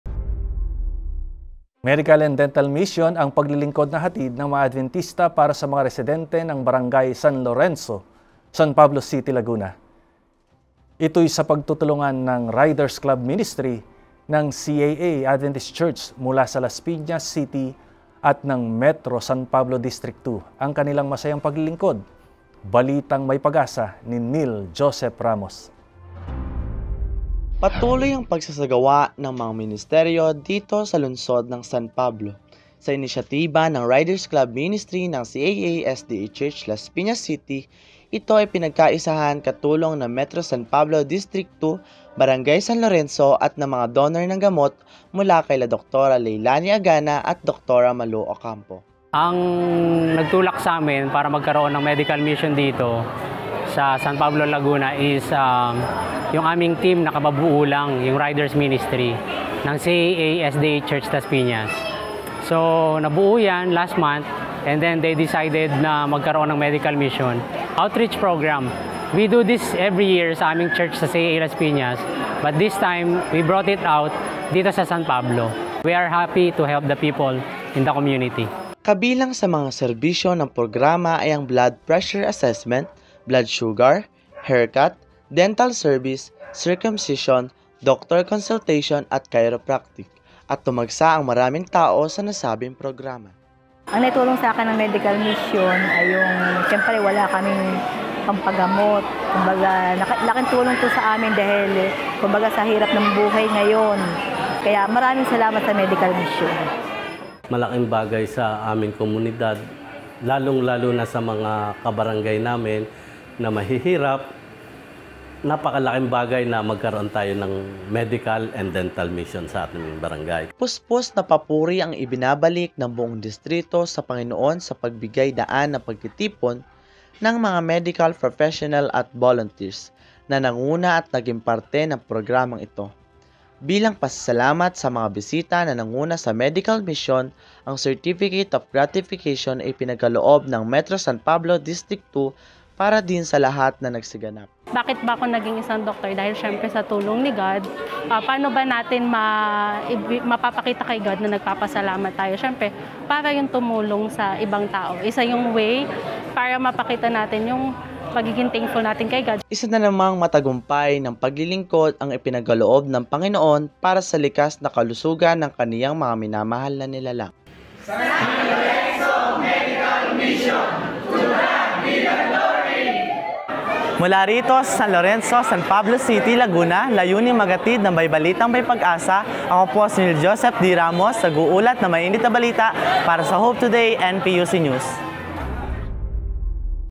Adventist Riders Club Ministry, Metro San Pablo District II nagsagawa ng Medical, Dental Mission
Medical and Dental Mission ang paglilingkod na hatid ng mga Adbentista para sa mga residente ng Brgy. San Lorenzo, San Pablo City, laguna.
Ito’y sa pagtutulungan ng Riders Club Ministry ng CAA Adventist Church mula sa Las Piñas City at ng Metro San Pablo District- II.
Ang kanilang masayang paglilingkod, balitang may pag-asa ni Niel Joseph Ramos…